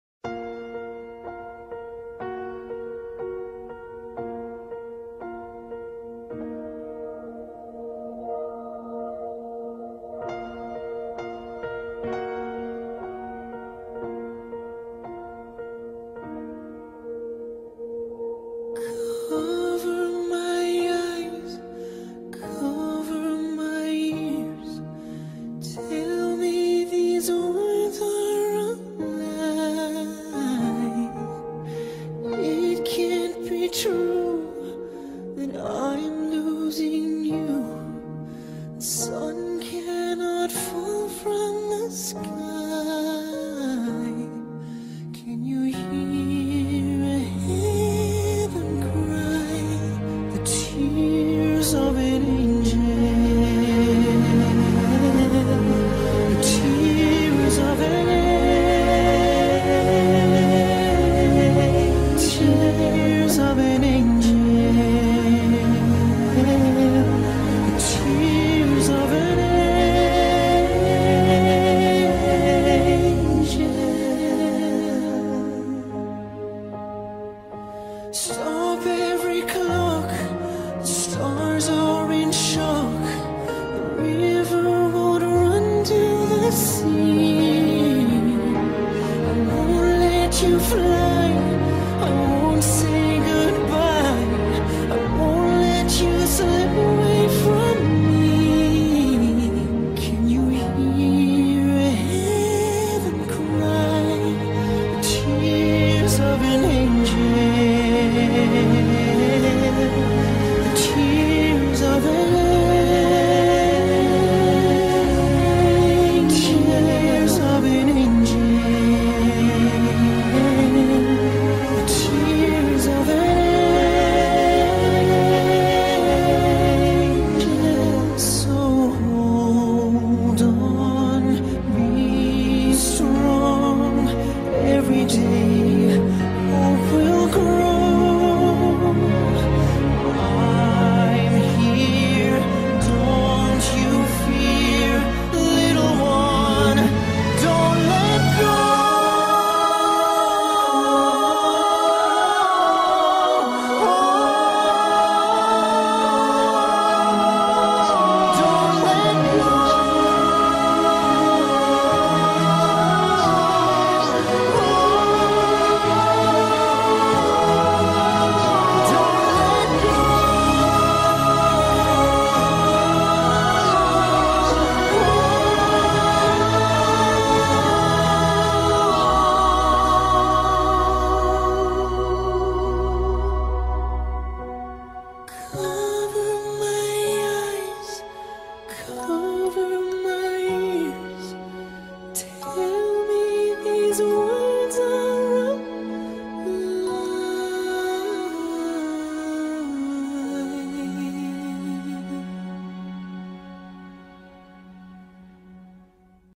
sophie quayle 9 years in heaven (2003 - 2007)
sophie was born on july 4 2003 and she died on october 6 2007 she is now 9 years in heaven rest in peace angel